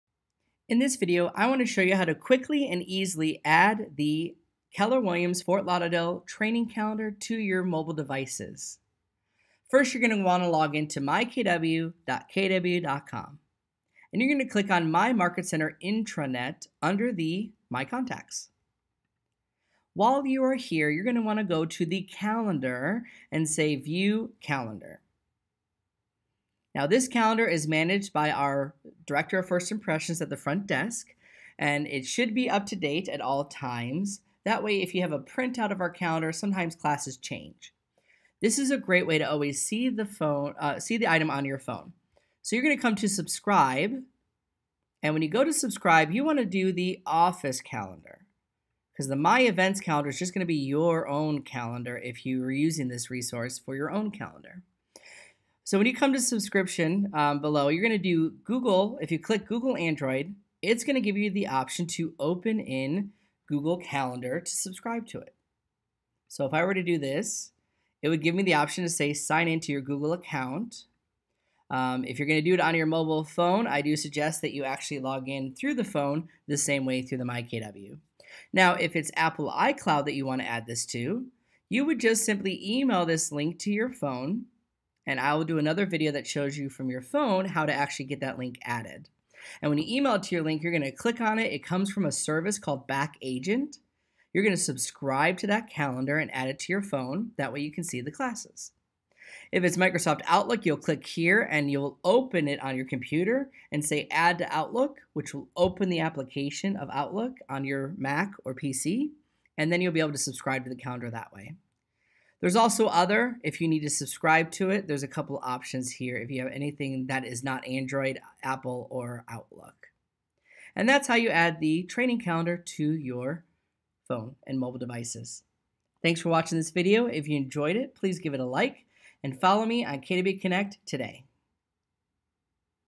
Subscribe to myMarket Center Intranet Calendar through BackAgent
This video was made for Market Center 711 in Fort Lauderdale, Florida, however the steps can be used for any market center that utilizes the mymarket center intranet calendar.  This service is provided by BackAgent.

You can add the training calendar to your Android Device, Google Calendar web based, iPhone/iPad and Mac, plus Outlook and more...

For more technology videos and Scripts around Technology please follow me on KW Connect Today!